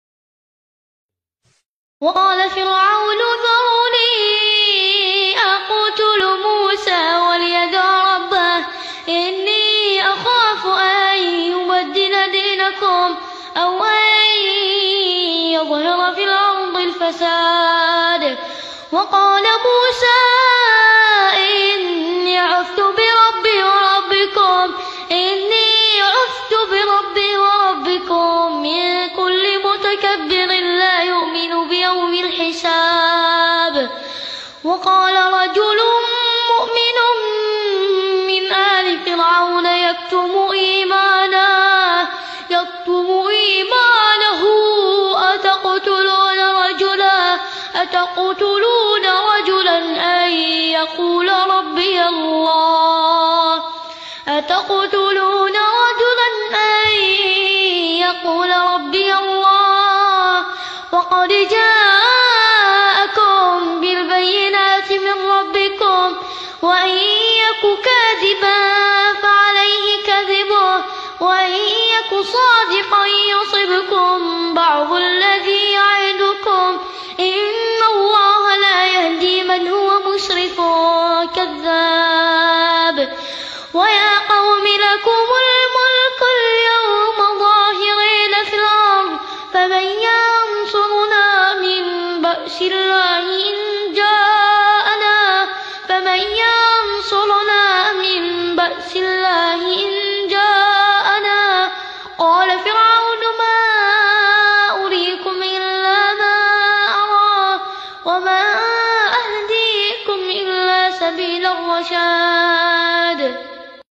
هذا الطفل الجميل يقلد الشيخ خالد الجليل بمنتهي الدقة 🥰
لو حابب تدعمنا